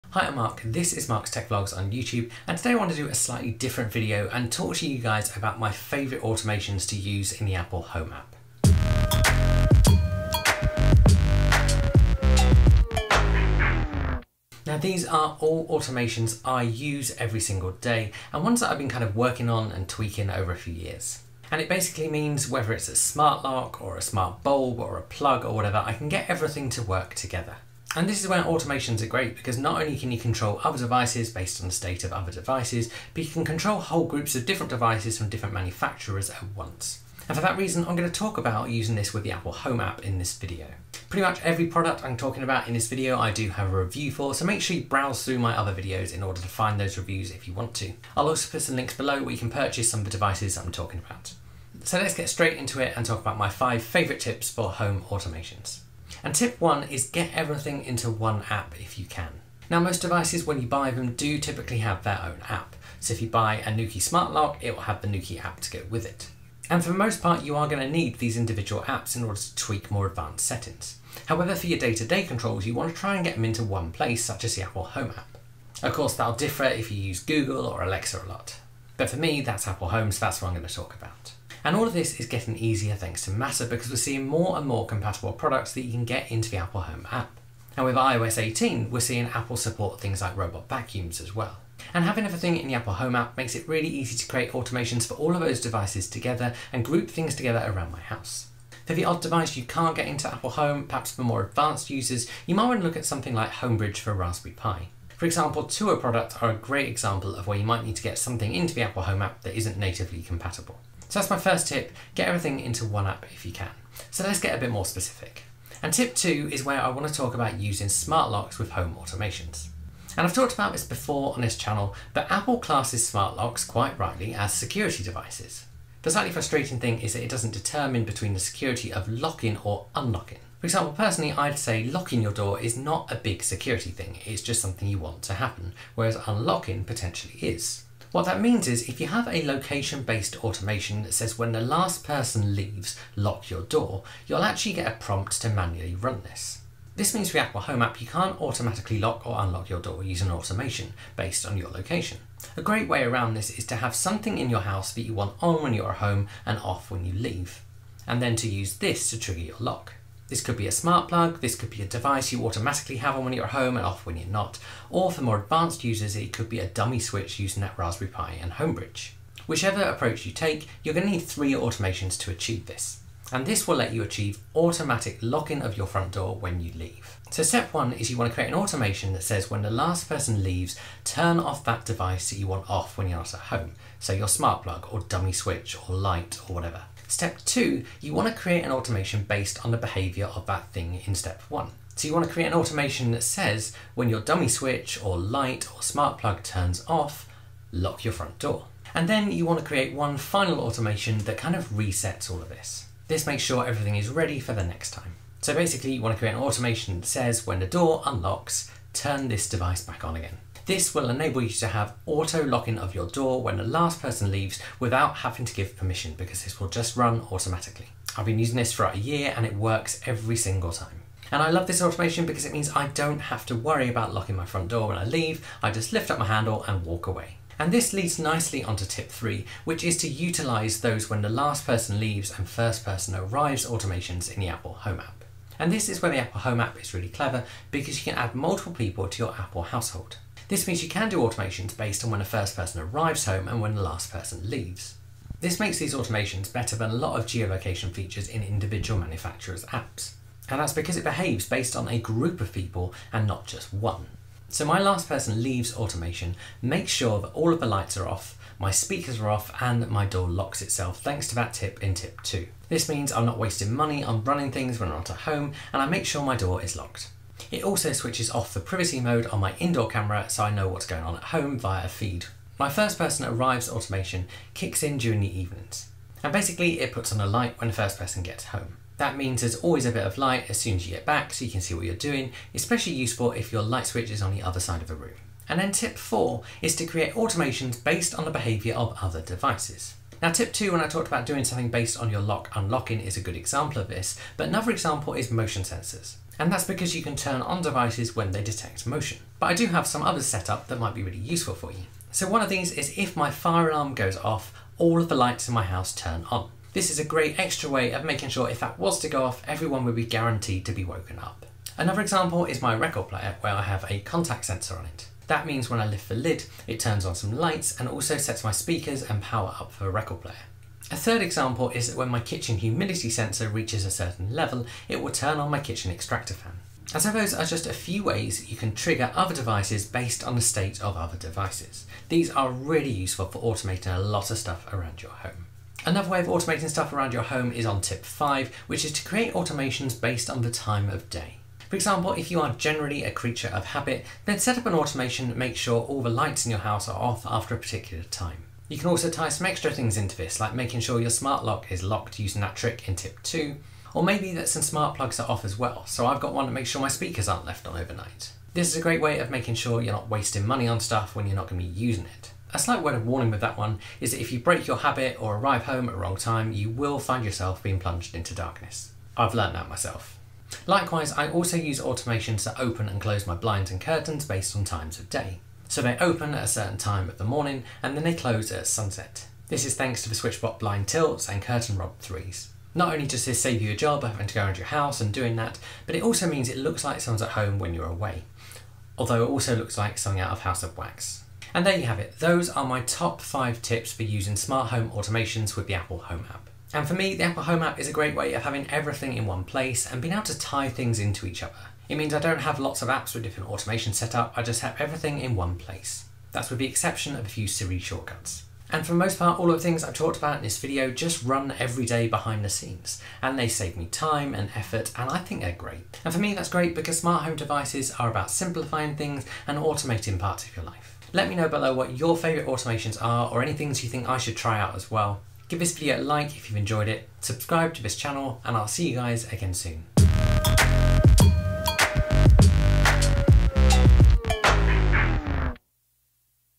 My Top 5 Apple Home Automation Tips!
Thanks to Apple HomeKit and Matter compatible products (with a hint of Raspberry Pi!) pretty much everything is in my Apple Home app and I use Apple Automations to automate loads of things around my house!

In this video I share my top 5 tips for automations using the Apple Home app

Follow Me On:

Found this helpful?